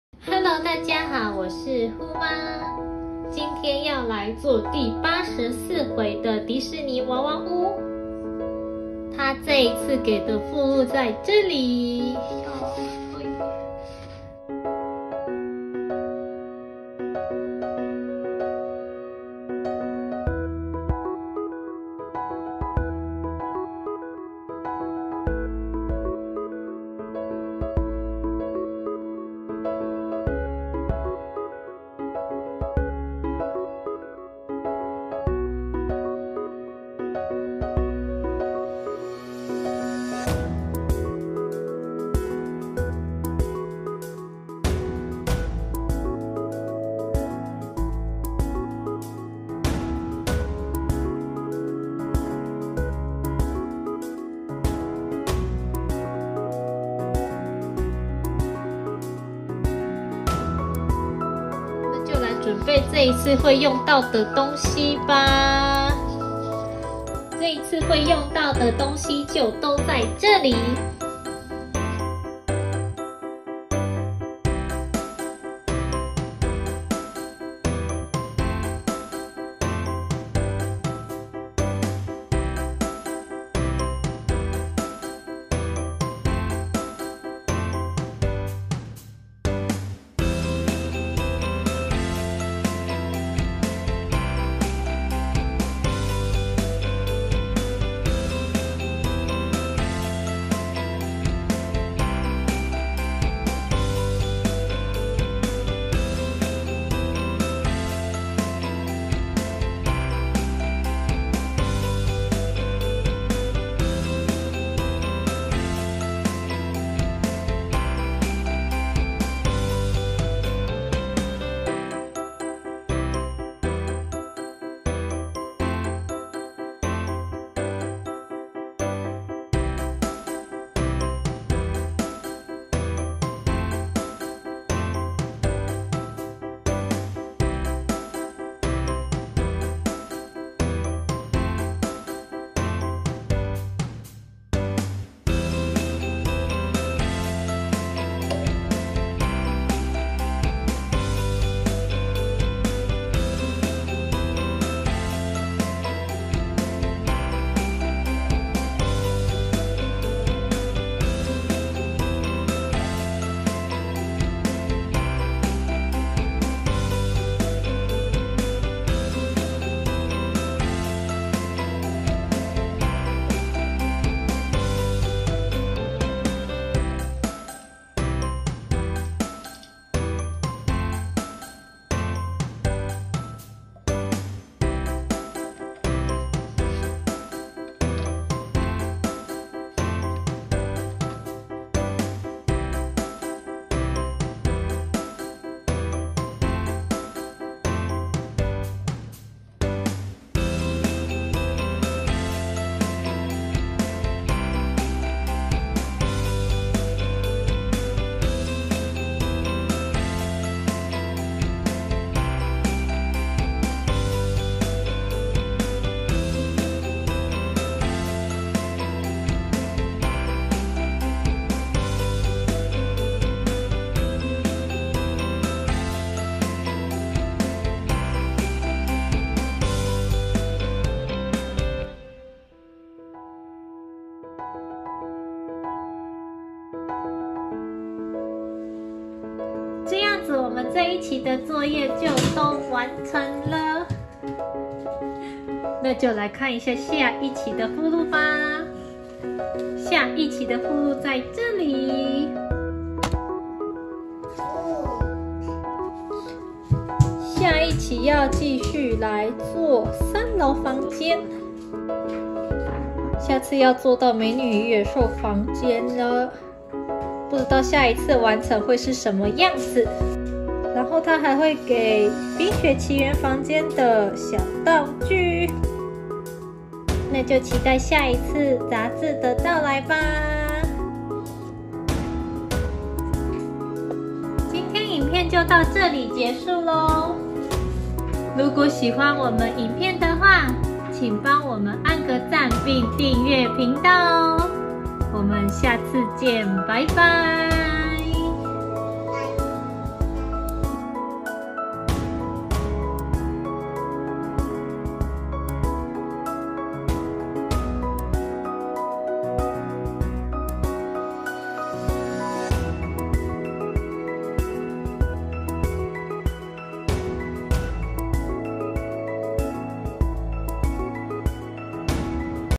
[FUFU手作館]台灣人挑戰製作日本手作雜誌迪士尼娃娃屋84
ディズニードールハウスをつくる-EP84
台湾人が日本の手作り雑誌を挑戦！

喜歡我們的影片的話